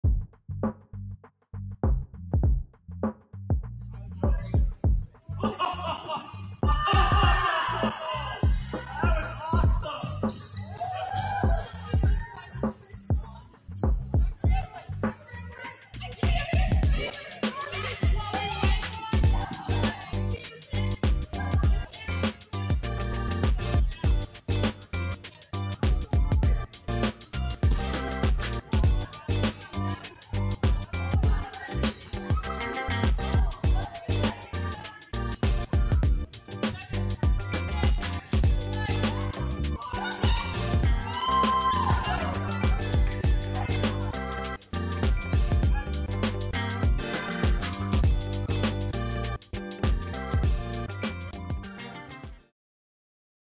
In The Box 1v1
Inspired by James Smith from The U of Strength. A warm-up activity we used to start our first practice in 2023.